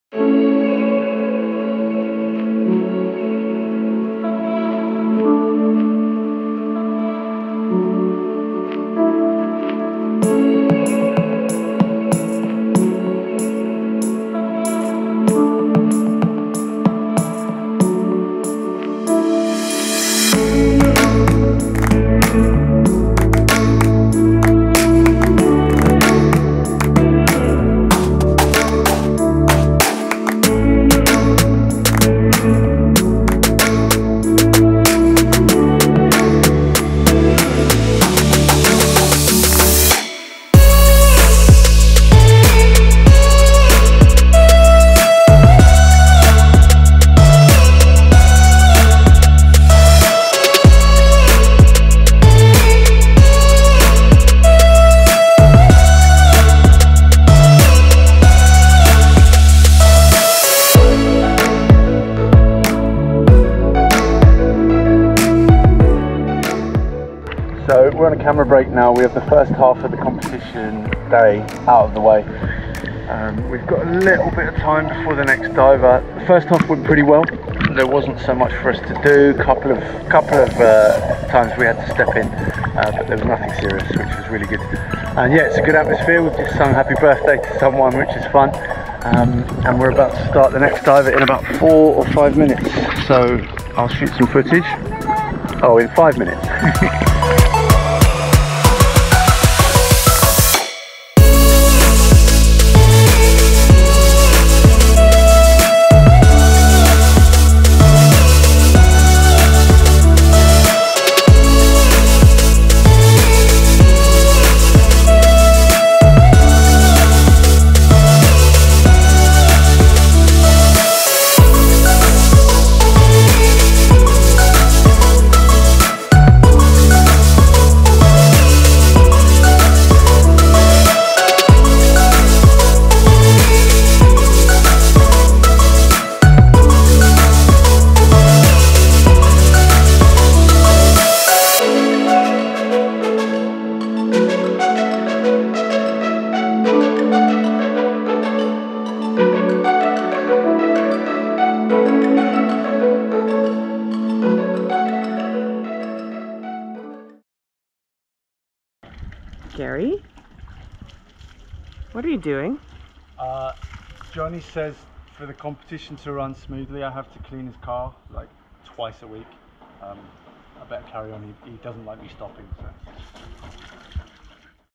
Diary of a Freediving competition safety diver
In November 2019 I worked as part of the safety team at the Blue Element Freediving Competition in Dominica for the second time, I made a wee video about it.